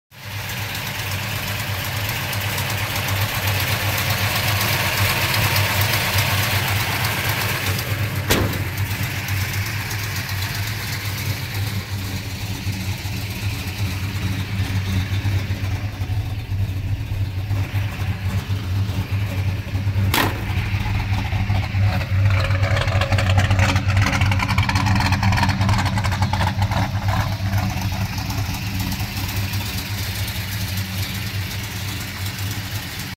1968 Dodge Coronet Autos Car For Sale in Topeka, Kansas
OldCarOnline.com 647189, - 1968 Dodge Coronet For Sale in Topeka, Kansas - A 1968 Coronet R/T H... These and more new and used Cars for sale online at OldCarOnline.com Classifieds.